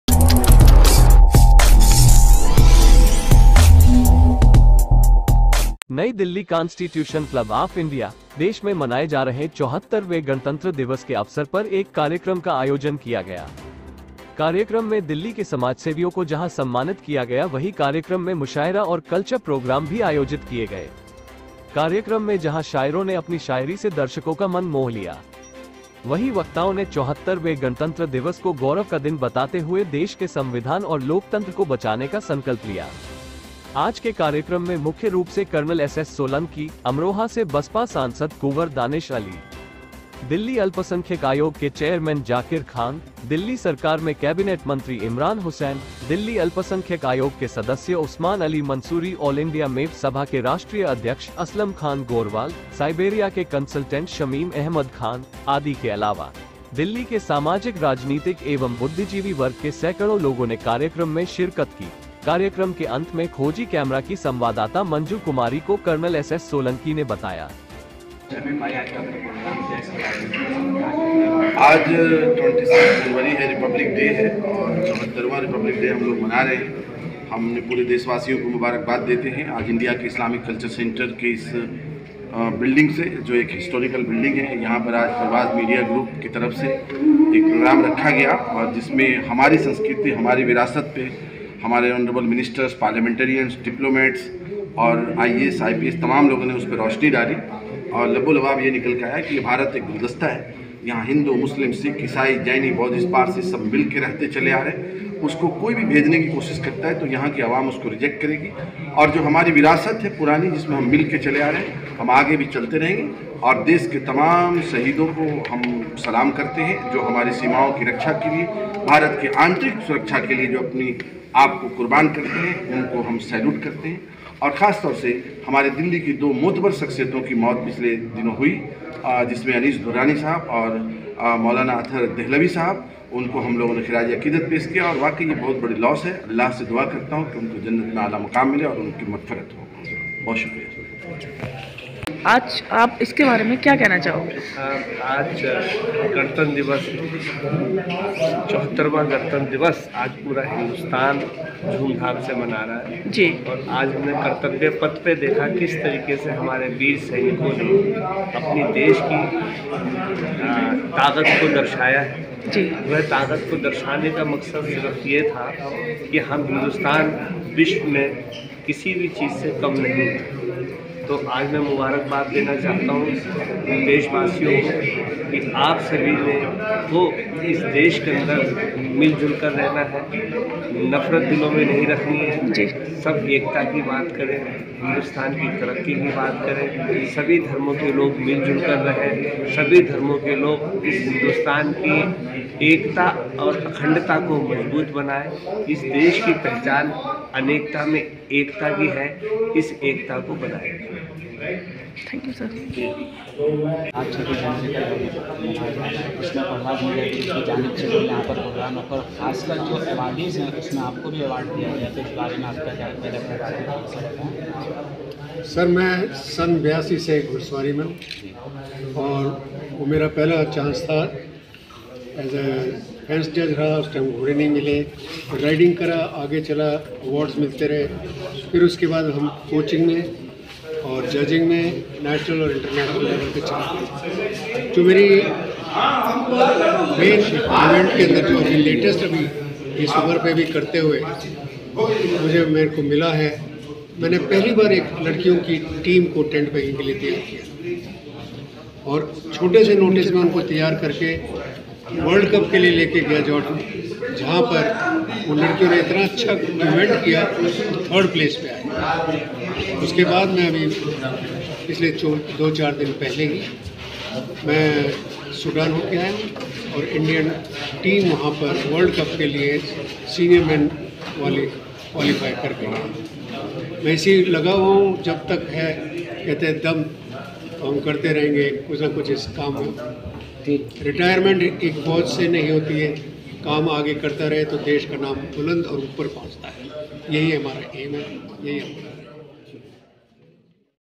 दिल्ली# कांस्टीट्यूशन क्लब आफ इंडिया में मनाया गया 74 वा गणतंत्र दिवस ,KHOJI CAMERA
दिल्ली कांस्टीट्यूशन क्लब आफ इंडिया में मनाया गया 74 वा गणतंत्र दिवस